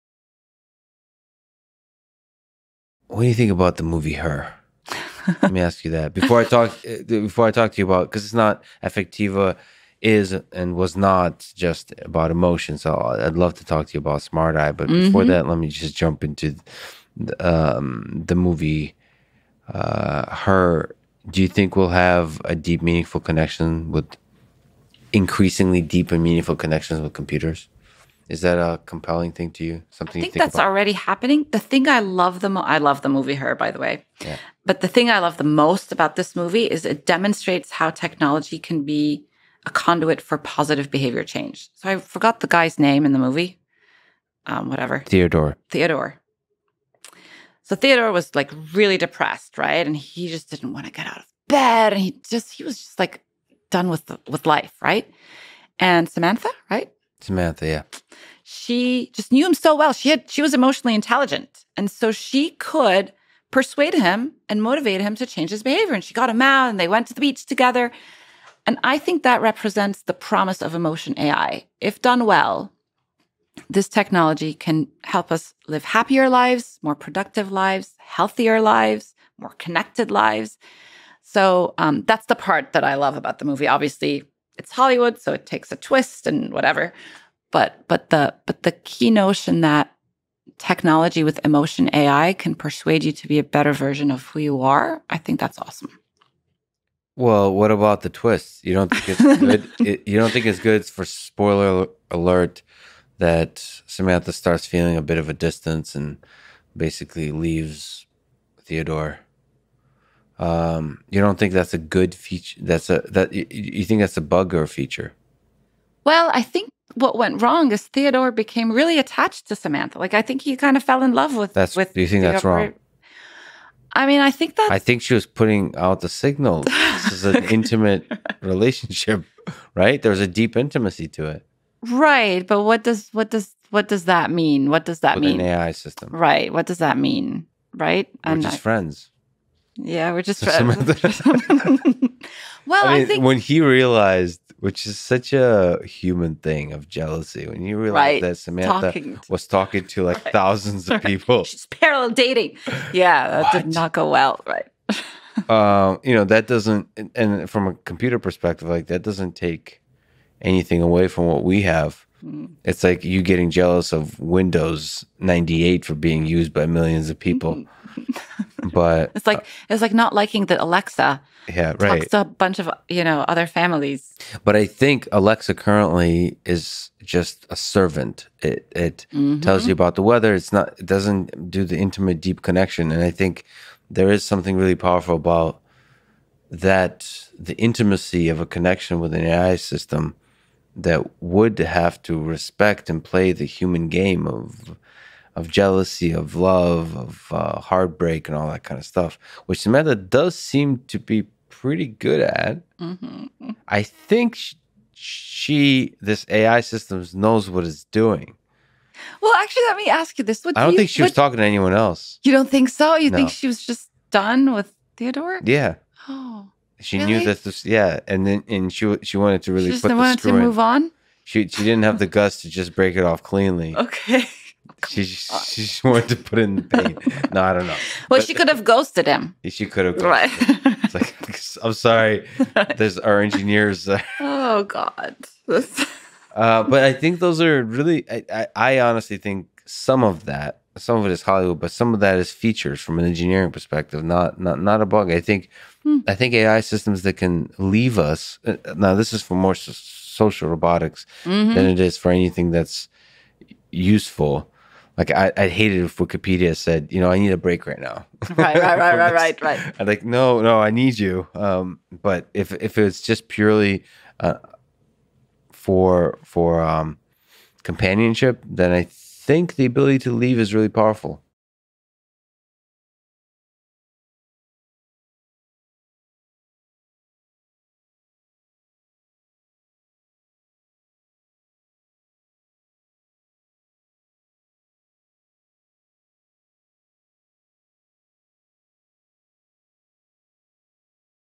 Lex Fridman on the movie Her
GUEST BIO:
Rana el Kaliouby is a pioneer in the field of emotion recognition and human-centric AI. She is the founder of Affectiva, deputy CEO of Smart Eye, and author of Girl Decoded.